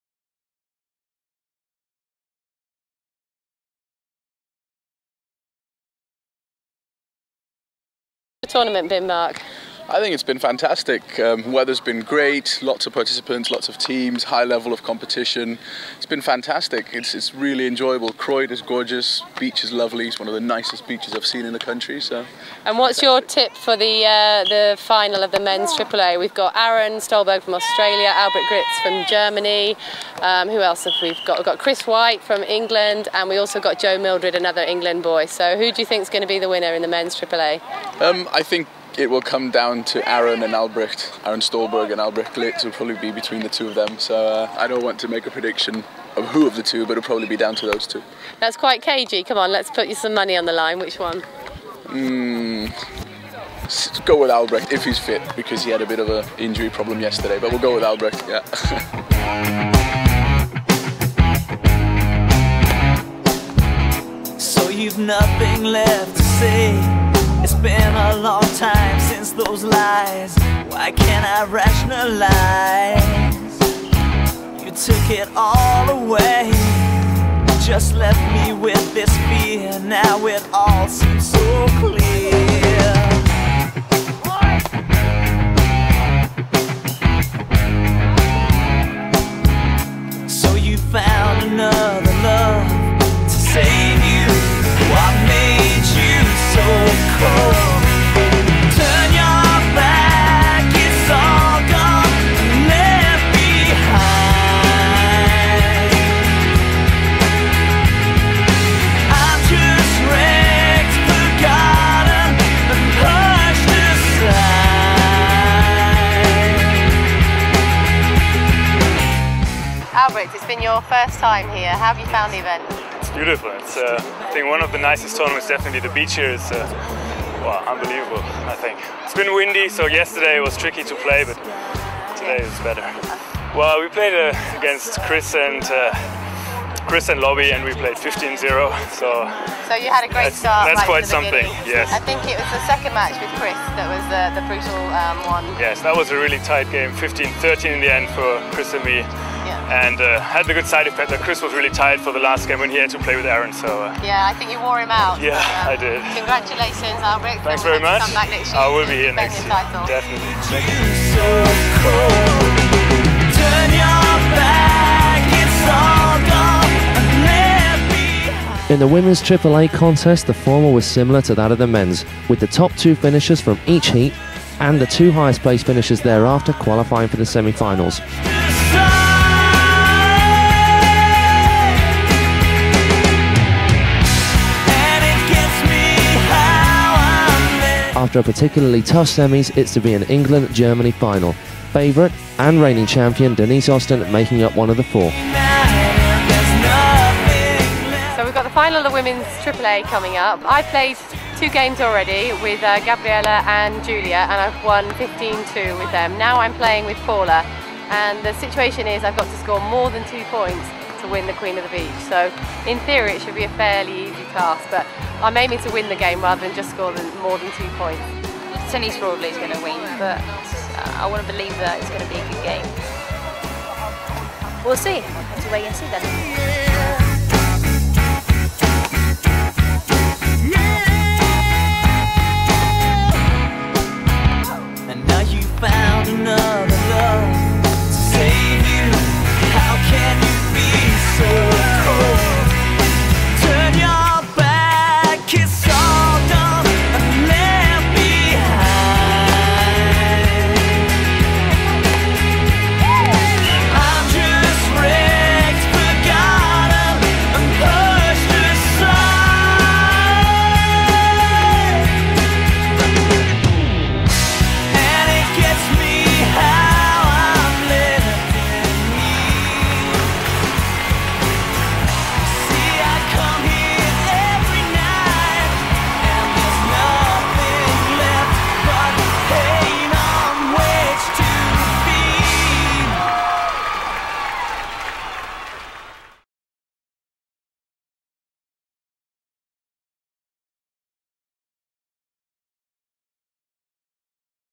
Goldcoast 2005 beach volleyball part 2
Beach Volleyball competitions at Croyde beach with commentary and interviews with Denise Austin and some of the previous winners who Denise was coaching. Part of the Goldcoast Oceanfest 2005 event with mens and womens beach volleyball competitions. This is part 2 of 2 with the finals of teh AAA contests and interviews with Albrecht Gliz. Features the music of Even Nine. Produced by maniac films ltd for the Goldcoast 2005 DVD.